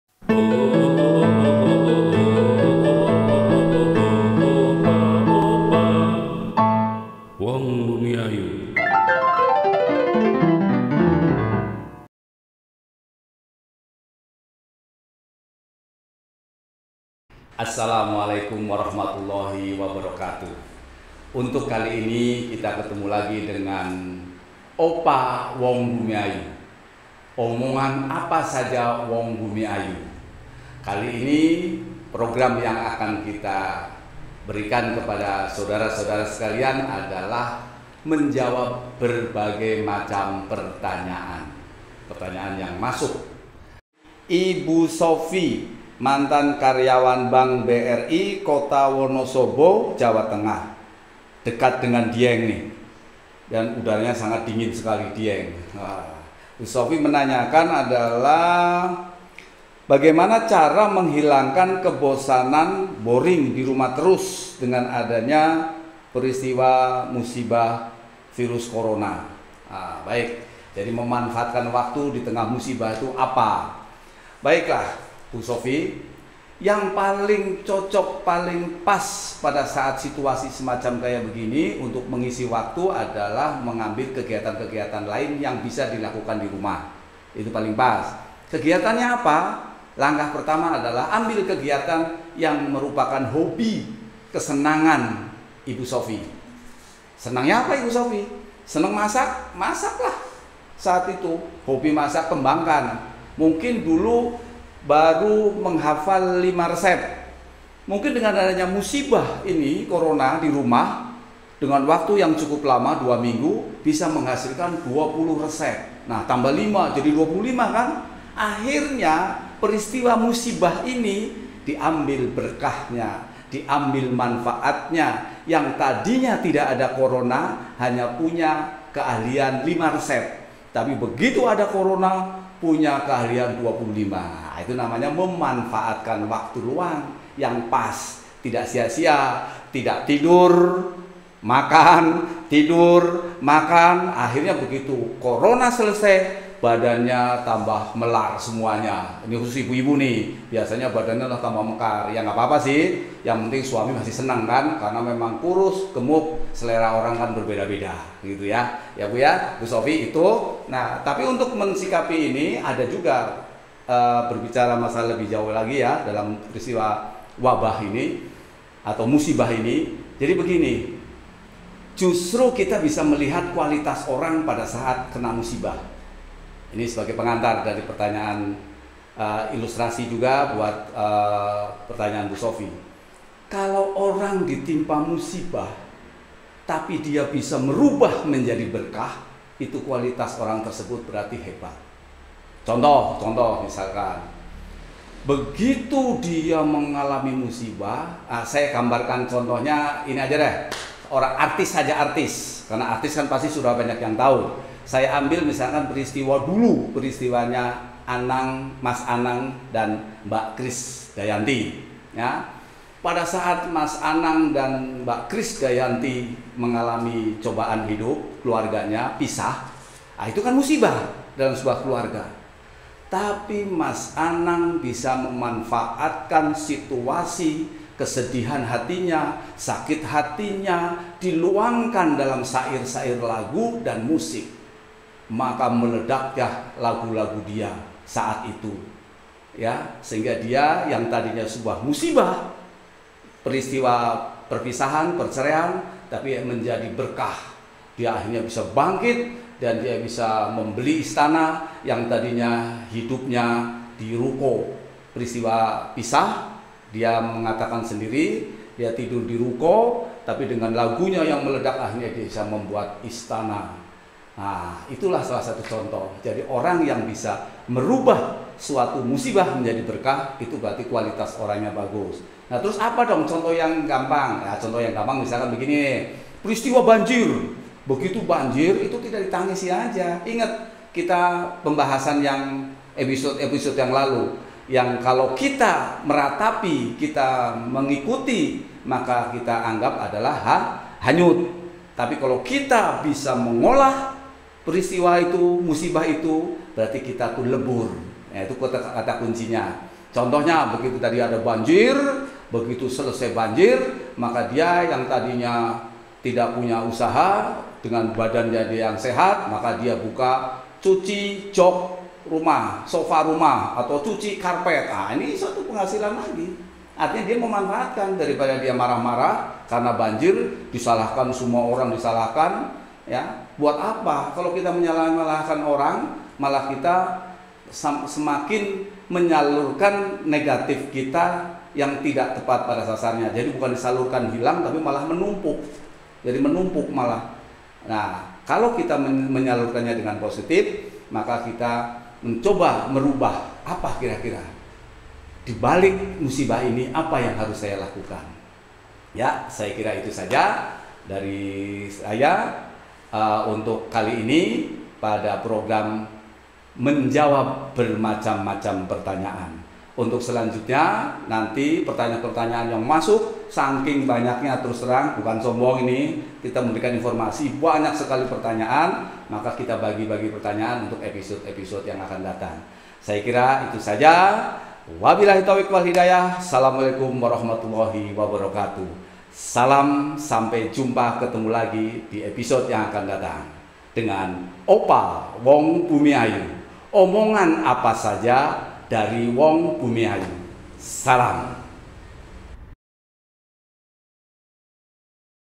Boring di rumah, OPA WONG BUMIAYU punya Jawabannya......
Bismillah.
Assalamualaikum Warahmatullahi Wabarakaatuh. Smoga kita semua senantiasa sehat dan penuh berkah. Aamiin.
Alhamdulillah kembali Opa Wong Bumiayu Menyapa dengan topik sangat bagus " Boring, bosen di rumah, karena wabah virus Corona " simak sampai tuntas. smoga bermanfaat.